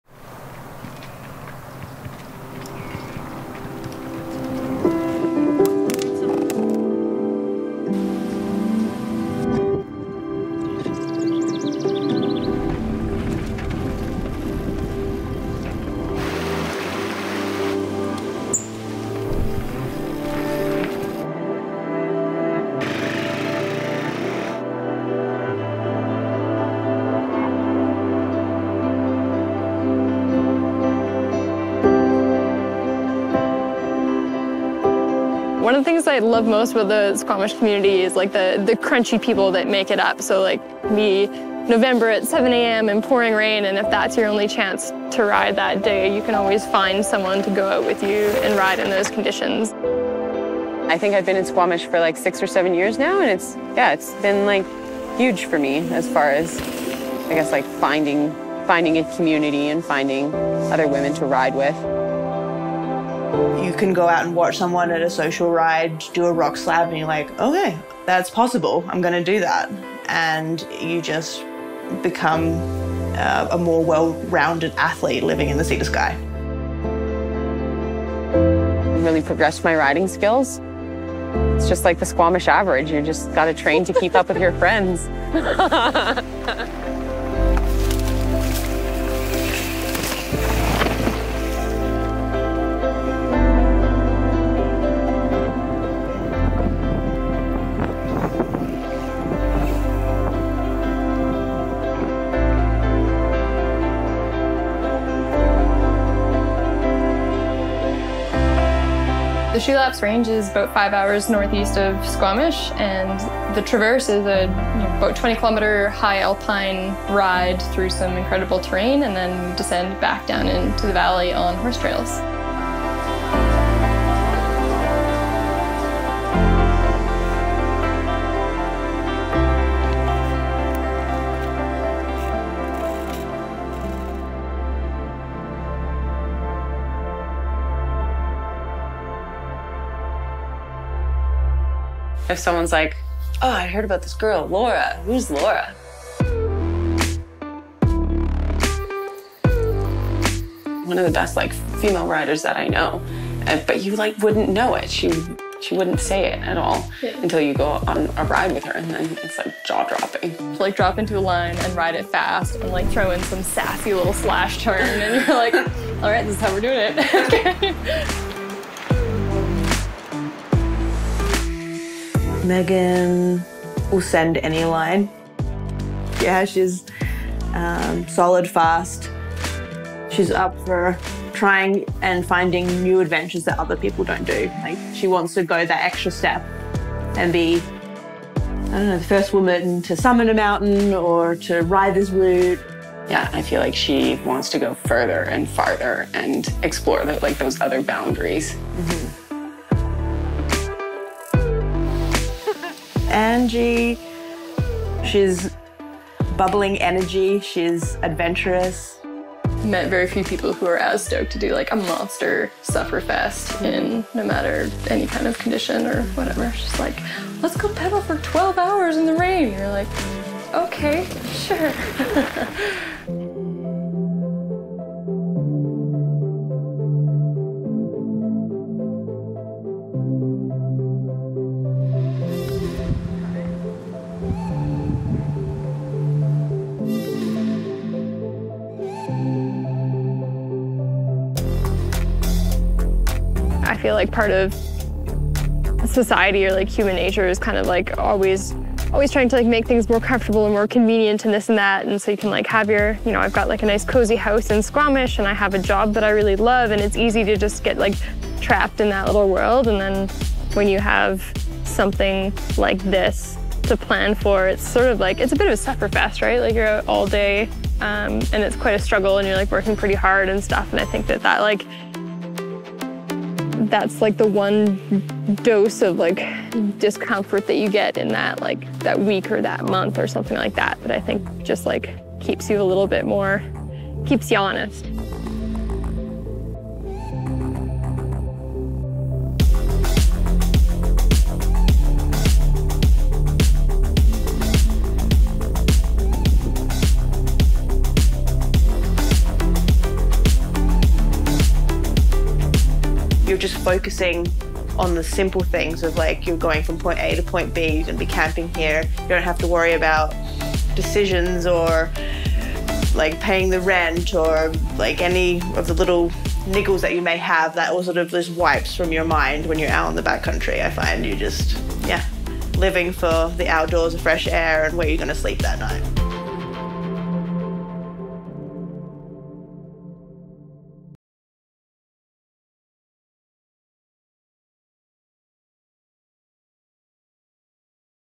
The Crunchy People | A 7mesh Women's Mountain Bike Series
In the parlance of our times, the Chilcotins, if you are to believe social media, have been done. Ridden, rated, reviewed and photographed to no end. They’re an awesome place to ride bikes, but some would say documented to death. But don’t believe all you read - there’s still a lot more to discover.

So in the spirit of adventure, we made some calls to local 7mesh ambassadors Laura Battista, Megan O’Brien and Angie McKirdy, mapped out a route and headed up to the often overlooked Shulaps Range for one last adventure before the alpine shut down.

We gratefully acknowledge that this journey took place on the unceded and traditional territories of the Tŝilhqot’in Nation.

Filmed and edited by Stirl and Rae Media Haus @stirlandraemediahaus

Watch Allies. Our project with the Indigenous Youth Mountain Bike Program.